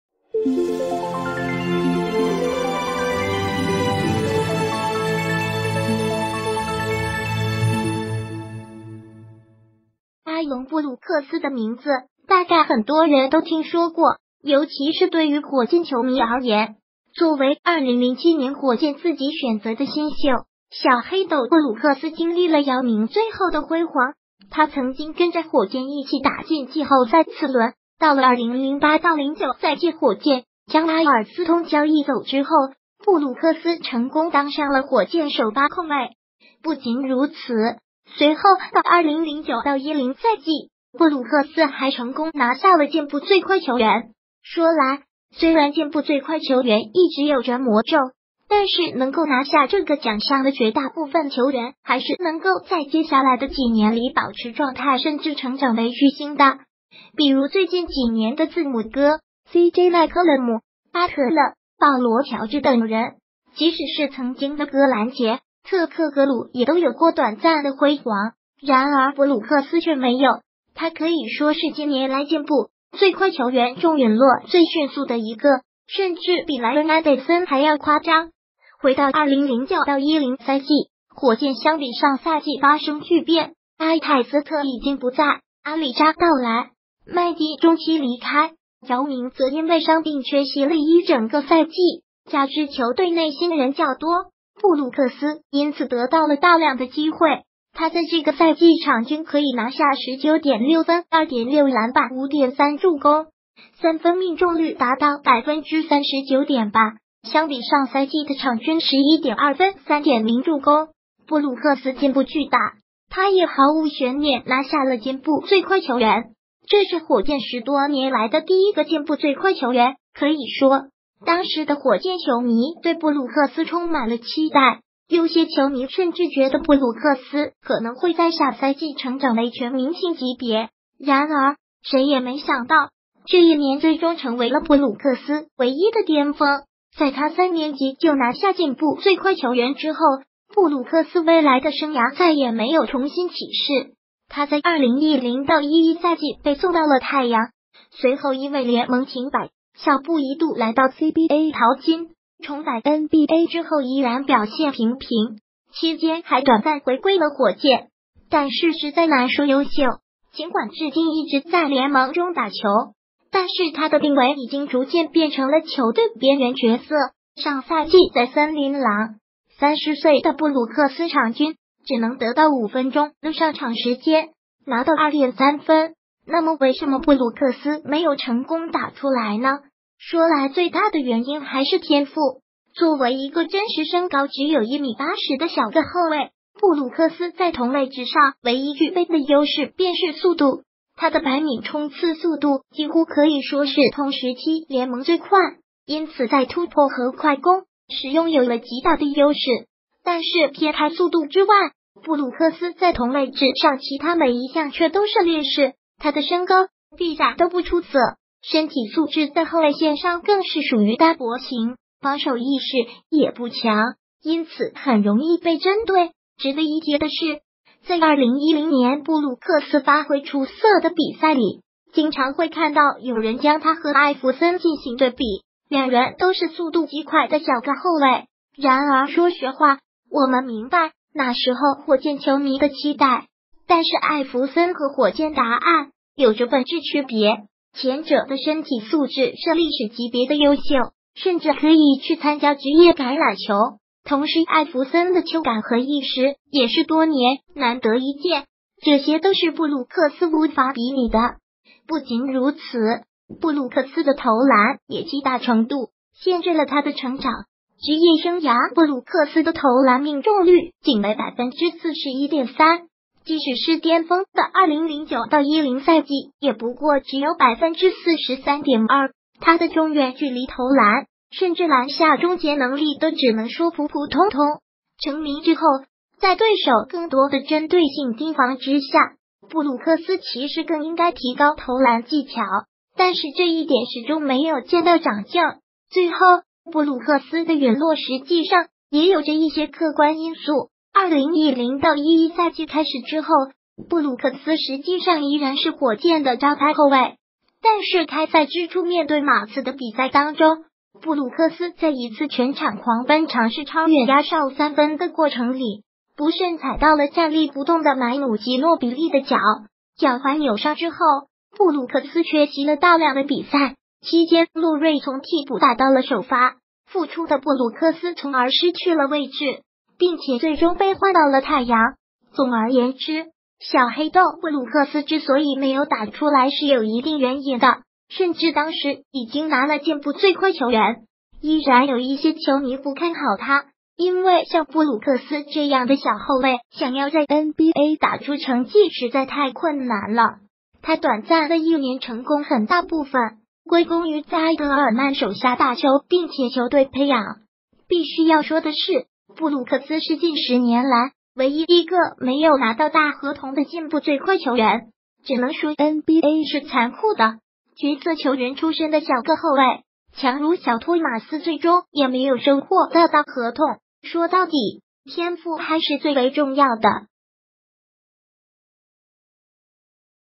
当年拿下进步最快球员后，布鲁克斯为什么没有成功打出来？
阿隆布鲁克斯的名字大概很多人都听说过，尤其是对于火箭球迷而言。作为2007年火箭自己选择的新秀，“小黑豆”布鲁克斯经历了姚明最后的辉煌，他曾经跟着火箭一起打进季后赛次轮，到了2008-09赛季火箭将阿尔斯通交易走之后，布鲁克斯成功当上了火箭首发控卫。不仅如此，随后的2009-10赛季，布鲁克斯还成功拿下了进步最快球员。说来，虽然进步最快球员一直有着“魔咒”，但是能够拿下这个奖项的绝大部分球员还是能够在接下来的几年里保持状态甚至成长为巨星的，比如最近几年的字母哥、CJ麦克勒姆、巴特勒、保罗乔治等人，即使是曾经的格兰杰、特克格鲁也都有过短暂的辉煌。然而布鲁克斯却没有，他可以说是近年来进步最快球员中陨落最迅速的一个，甚至比莱恩安德森还要夸张。回到2009-10赛季，火箭相比上赛季发生巨变，阿泰斯特已经不在，阿里扎到来，麦迪中期离开，姚明则因为伤病缺席了一整个赛季，加之球队内新人较多，布鲁克斯因此得到了大量的机会。他在这个赛季场均可以拿下19.6分2.6篮板5.3助攻，三分命中率达到39.8%，相比上赛季的场均11.2分3.0助攻，布鲁克斯进步巨大，他也毫无悬念拿下了进步最快球员，这是火箭十多